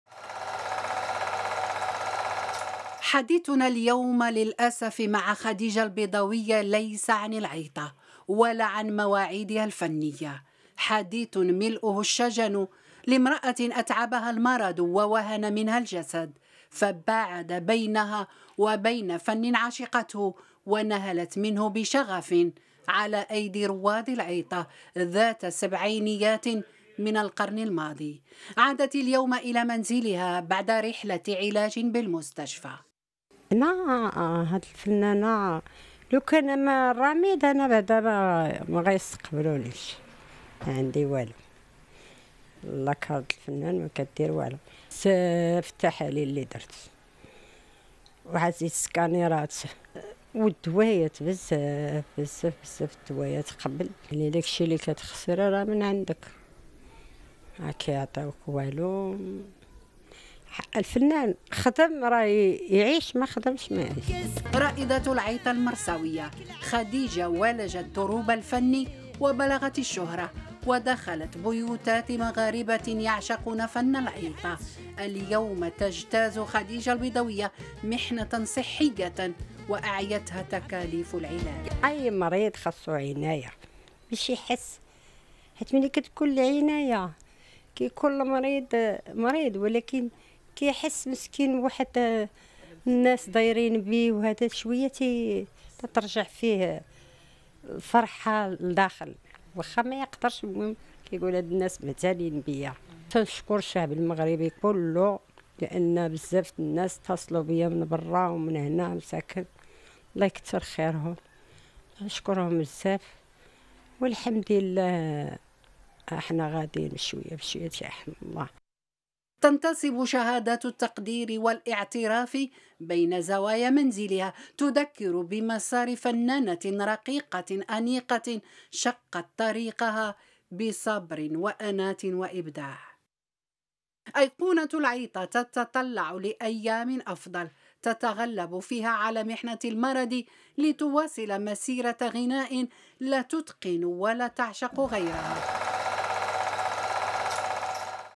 ذكرى الراحلة خديجة البيضاوية .. معاناة فنانة استثنائية
ترجلت فنانة العيطة خديجة البيضاوية عن صهوة الحياة بعد صراع مع مرض السرطان، لتفقد الذاكرة الفنية المغربية، واحدة من رموز العيطة المرساوية.
تتوقف كبسولة "ذكرى" اليوم السبت 15 أكتوبر 2022 عند معاناة خديجة البيضاوية مع مرض السرطان قبل أن توافيها المنية في الساعات الأولى من صباح اليوم.
وتتحدث رائدة العيطة المرساوية خديجة البيضاوية عن صراعها مع المرض، موجهة شكرها للشعب المغربي على اهتمامه بصحته وكل من ساندها في محنتها.

يرجى الاشتراك..